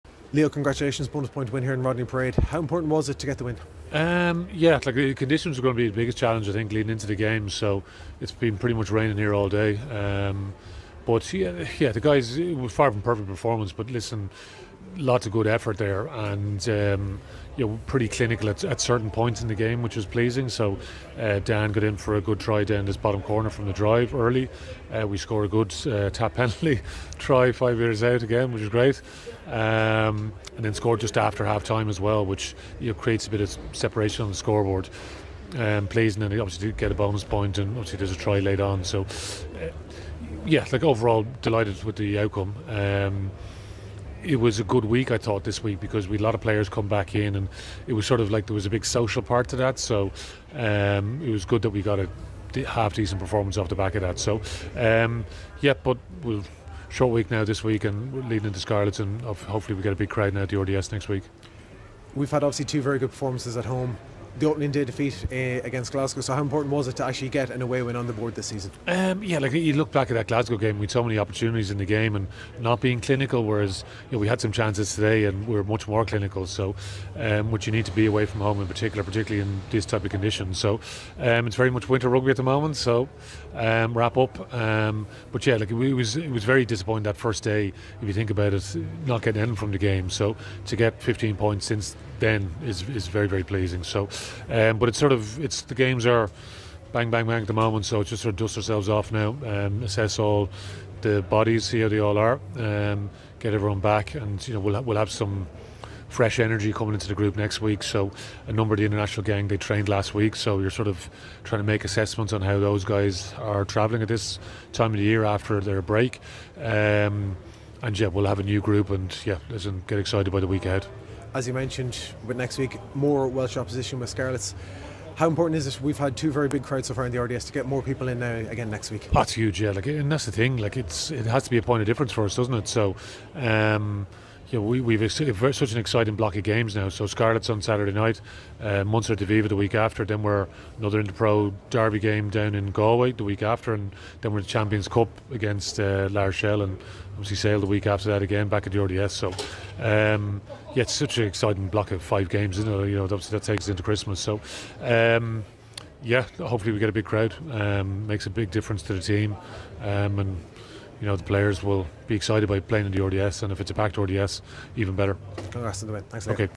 Leo Cullen post match | Dragons v Leinster | 12 November 2023
Leo Cullen says more international players will be available for our game against Scarlets next week. 💪